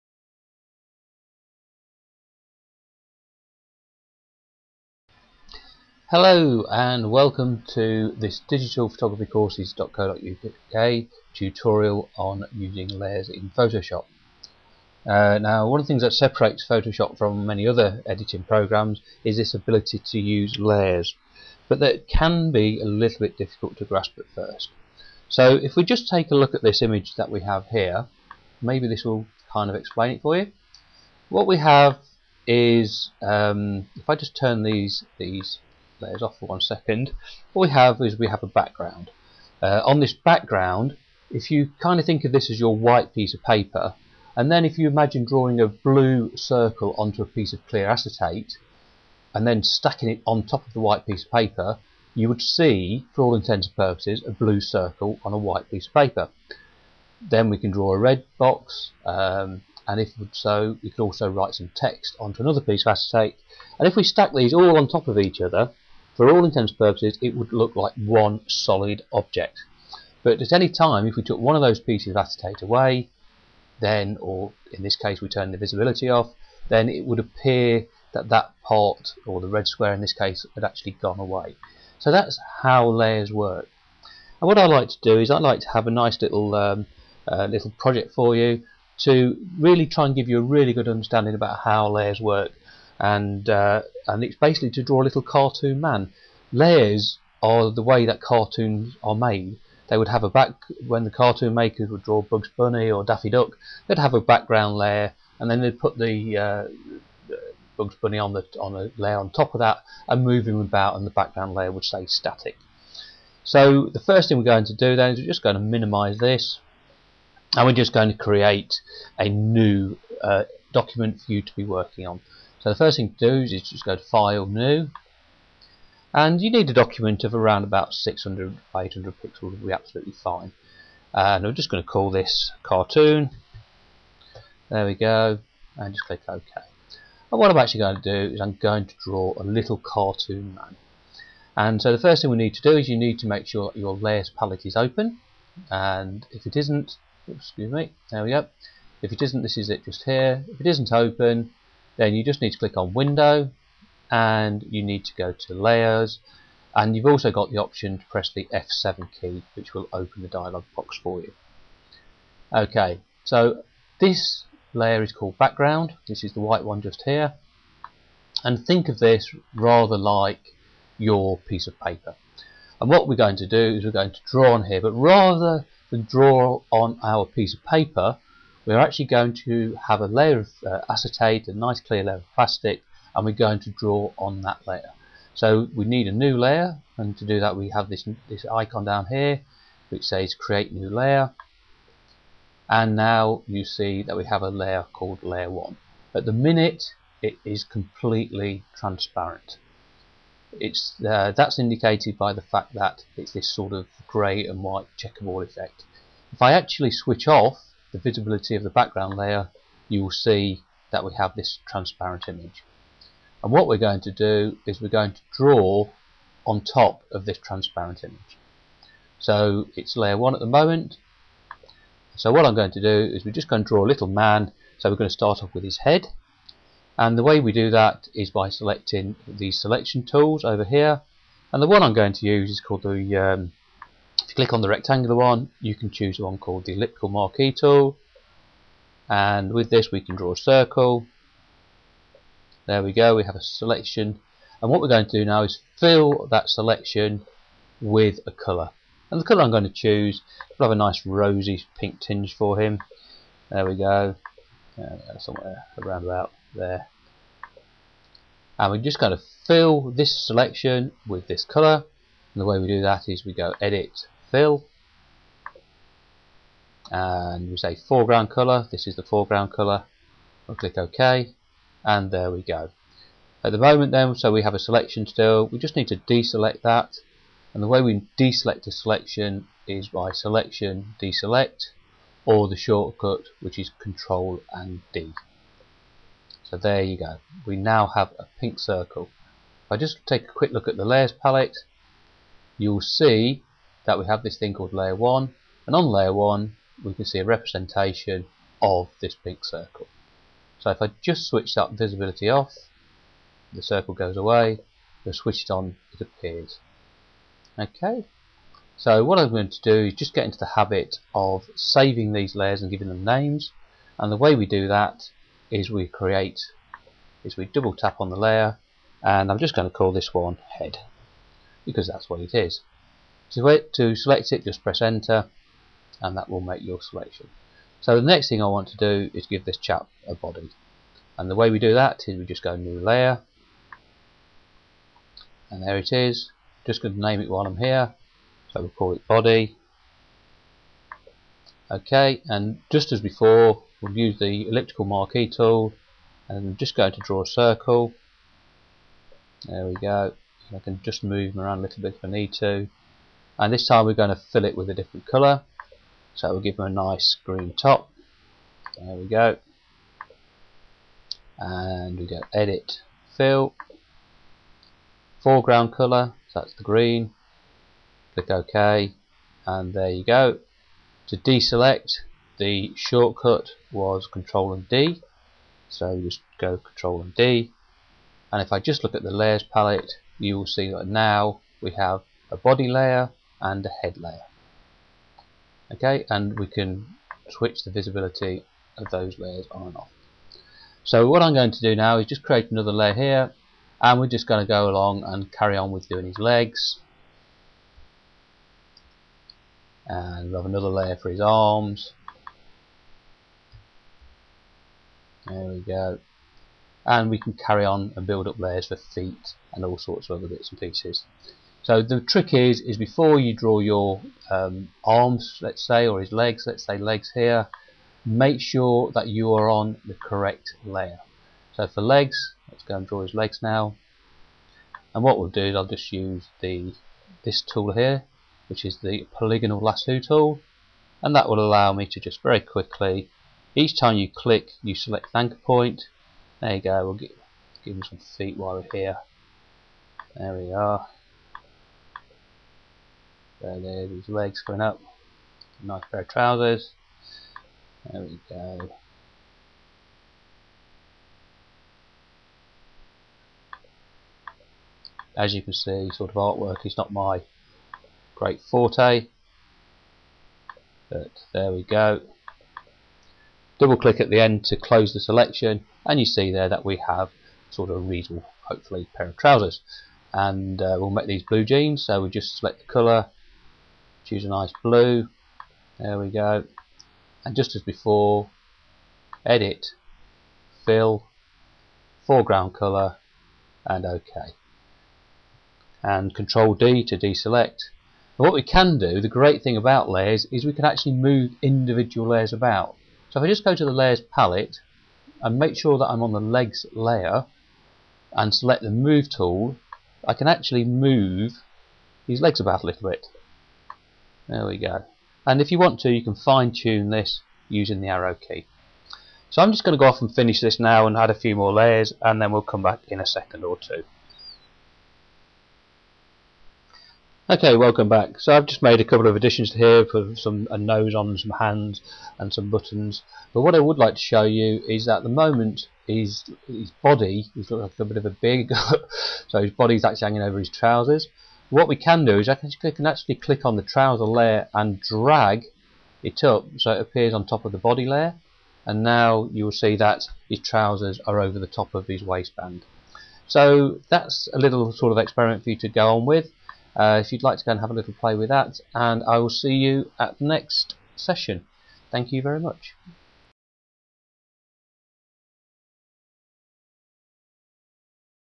Using Layers and the selection tools in Photoshop - tutorial 1
Learn how to use layers and the selection tools in this Photoshop tutorial from Digital Photography Courses.co.uk. You will learn how to move a layer from one picture into another. Part 1 of 3